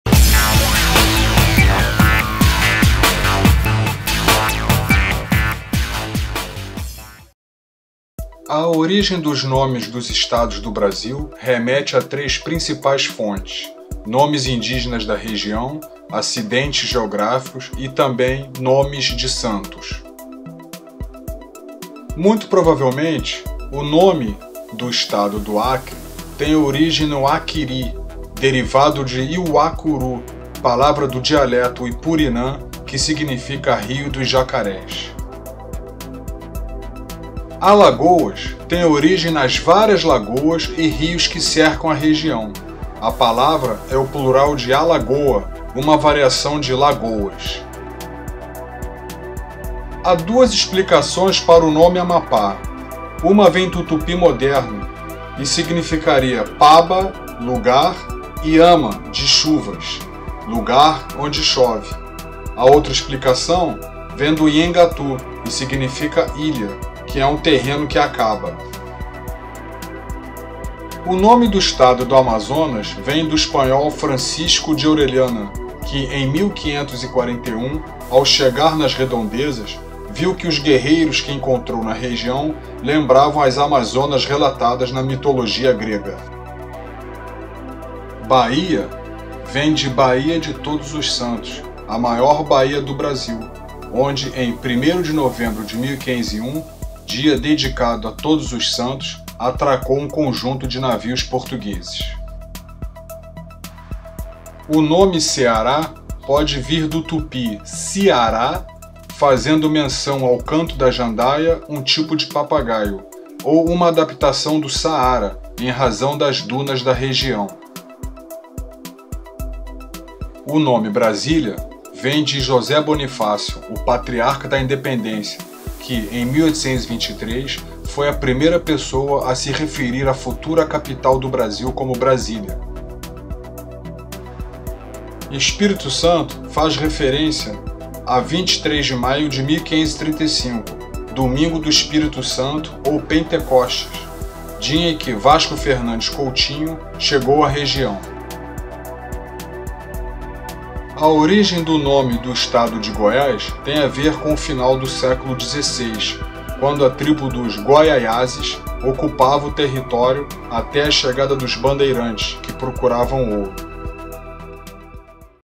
✅ A origem dos NOMES DOS ESTADOS DO BRASIL [Parte #1]
A origem dos nomes dos estados do Brasil remete  a três principais fontes: nome de tribos indígenas da região, acidentes geográficos e nomes de santos.
Por trás dos nomes dos estados do Brasil há uma história, por vezes controversa, mas que nos dá uma noção de como cada estado foi nomeado.

Se gostou do vídeo dá um like, compartilhe e assine nosso canal!!